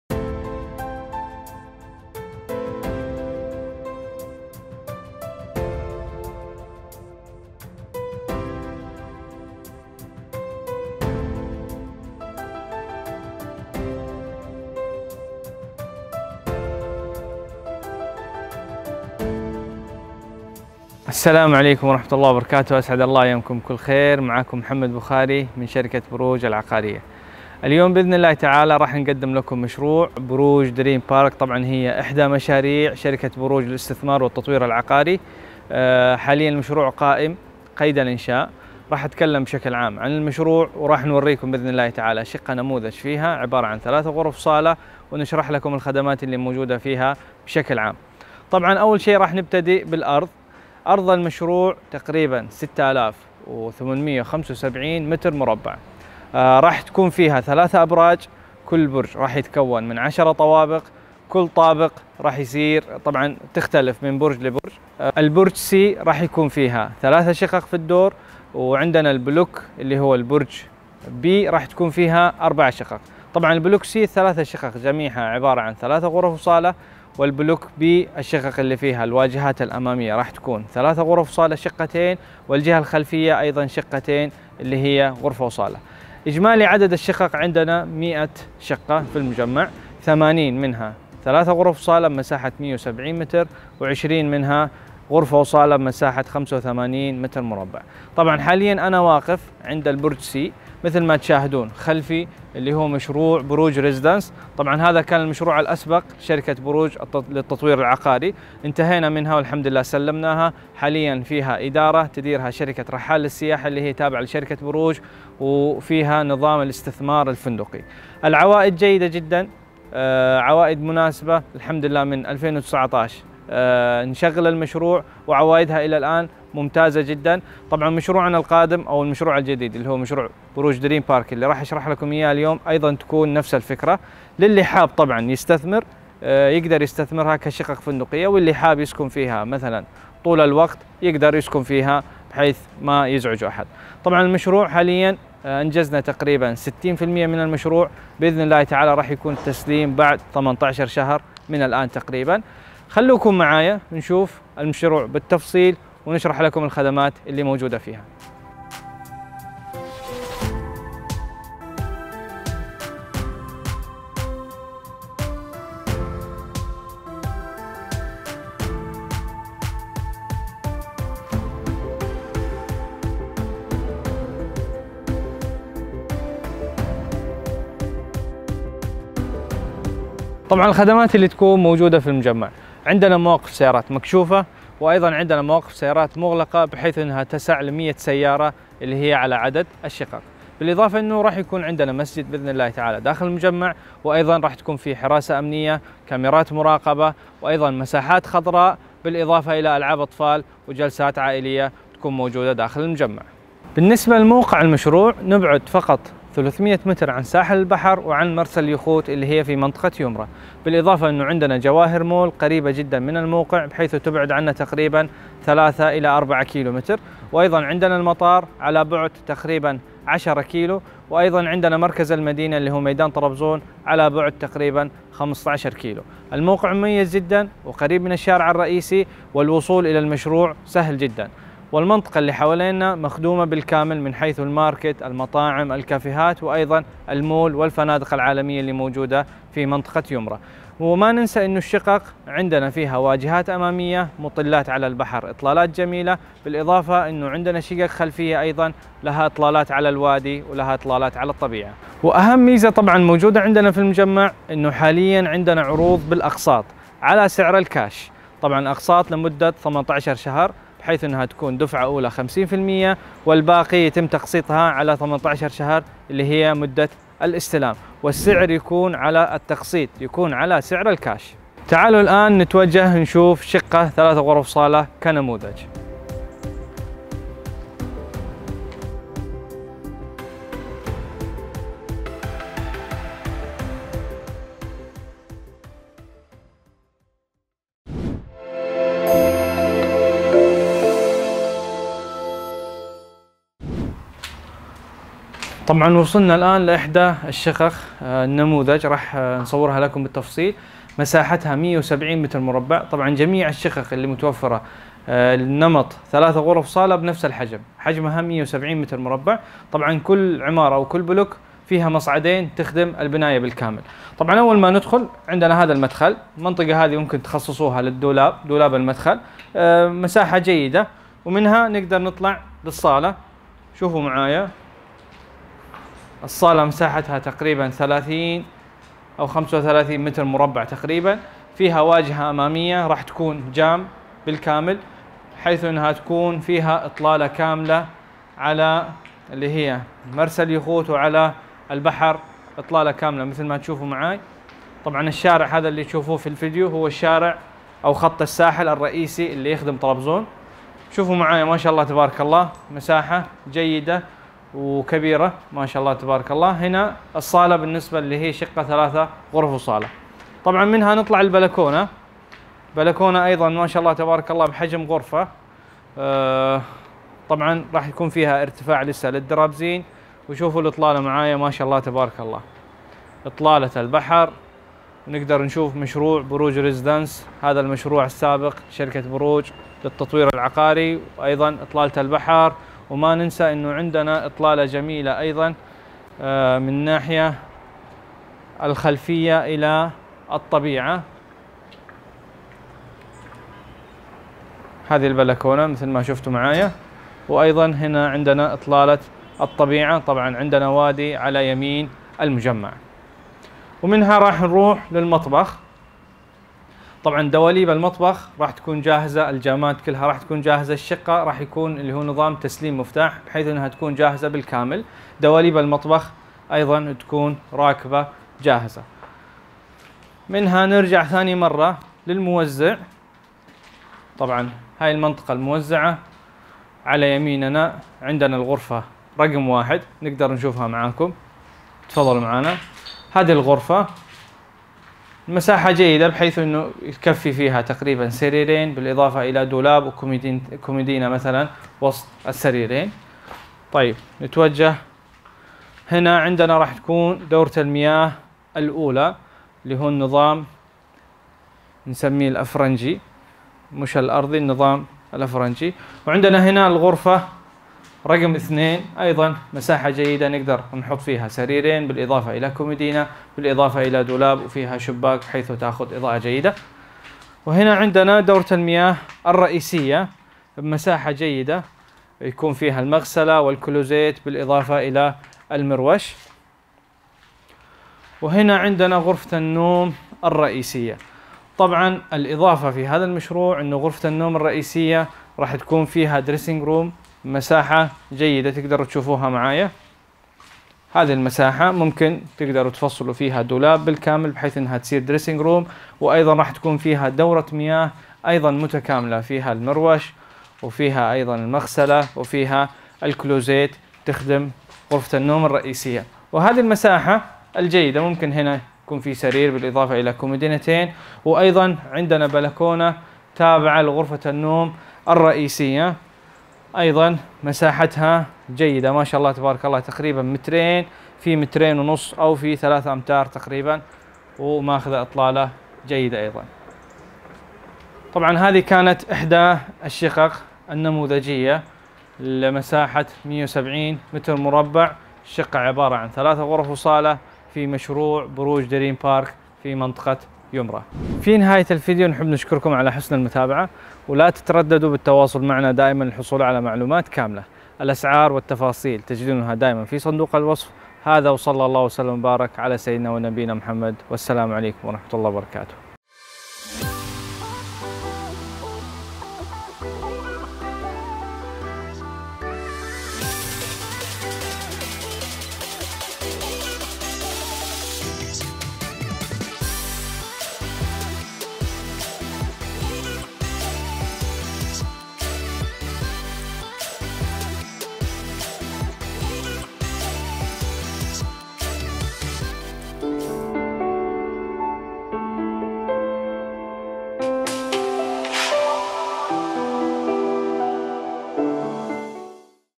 فرصة تملك شقق في طرابزون تركيا -عرض خاص50% | مشروع بروج دريم بارك فرصة استثمارية مع شركة بروج
مشروع دريم بارك يقع في قلب مدينة طرابزون الجديد
وبالتحديد منطقة يومرا التي تعد من اكثر المناطق تطورا
والتي تم انشاء اكثر من 8 فنادق عالمية

* يبعد المشروع عن شاطئ البحر فقط 300 متر وعن مول جواهر وفندق النوفوتيل 5 دقايق فقط  ويبعد عن المطار 10 دقائق فقط .

*ويمكن الوصول لمركز المدينة  في  15دقائق
والى جنة الله في ارضه قرية اوزنجول  في 45 دقيقة فقط .

* تفاصيل المشروع :

- مساحة الارض للمشروع ( 6900) متر مربع .
- يتكون من ( 3 ابراج  سكنيه مطلة على البحر ) .
-  كل برج ( 10 ) دور ( مجموع 100 شقة )  .

* تتكون الوحدات السكنية من نوعين :

 ١- ( 3 غرف نوم + صالة + 3حمامات + مطبخ + 2بلكونه ) مساحة الشقة( 170 متر مربع ) .

٢ - ( 1 غرفة نوم + صالة مع مطبخ امريكي+ حمام + بلكونة 10 متر مربع ) مساحة الشقة 85 متر مربع

المميزات في المشروع /
-  ما يميز المشروع بالدرجة الاولى اطلالتها السحرية على البحر والطبيعة .
-  مسجد خاص بالمجمع
-  صالة جيم ولياقة
- مواقف سيارات مغلقة ( موقف لكل شقة )
-  جلسات خارجية مميزة .
-  ملعب كره سلة وقدم .
-  شلالات صغيرة .
-  العاب اطفال .
-  مساحات خضراء .
-  ممشى خاص بالمجمع .
-  بوابه للمجمع وحراسه امنية وكاميرات مراقبة .
- مصعدين لكل عمارة .
-  صالة اجتماعات .
- ميني سينما .
- غرفة اجتماعات للمجمع .
-  على 4 شوارع .
- قريبة من ( شوك ماركت - محطة شل ) وكل الخدمات
-  5 دقائق عن جامعة اوراسيا
- 5 دقائق عن مول جواهر
- مقابل مرسى اليخوت وشاطئ يومرا .

🔥مميزات الشقق /
تتميز الشقق في مشروعنا بالتالي :
- دولاب مطبخ  .
- دواليب دورات المياة
-  باركيه .
- سيراميك درجة اولى
- شفاطات في دورات المياة
- شطافات في دورات المياة
- توصيل الماء الحار لكافة دورات المياة

✅ ما يميز المشروع اكثر انه يمكن الشراء والاستثمار للشقق عن طريق تأجيرها على من خلال شركة رحال للسياحة بعوائد تصل الى 12٪؜ .

📍موقع المشروع على قوقل ماب /
‏İşaretli Yer

تبدا الاسعار من ٣٥ الف دولار لغرفة وصاله
والاسعار تبدا من ٧٢ الف دولار ل ٣ غرف وصاله
وتختلف الاسعار حسب الدور

رقم شركة بروج /
رابط الواتساب المباشر للتحدث معي

عقارات_اسطنبول#
عقارات_تركيا#
الاستثمار_في_تركيا#
شقق للبيع في تركيا
شقق_تركيا#
عقارات_طرابزون#